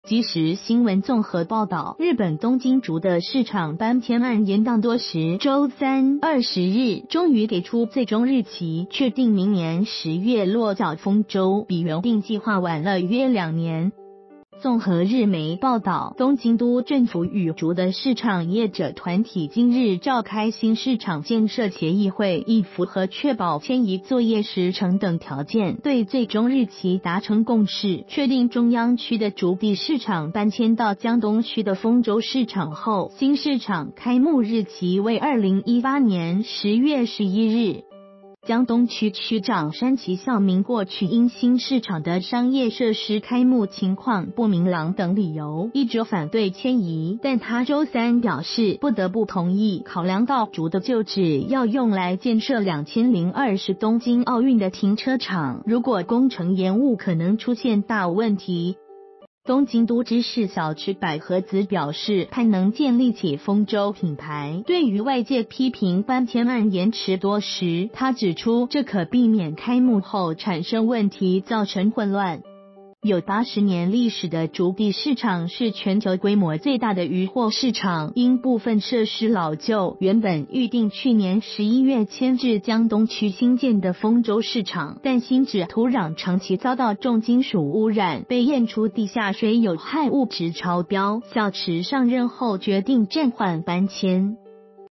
東京築地市場落腳豐洲 明年10/11開幕
〔即時新聞／綜合報導〕日本東京築地市場搬遷案延宕多時，週三（20日）終於給出最終日期，確定明年10月落腳豐洲，比原定計畫晚了約2年。

發表評論評論如下。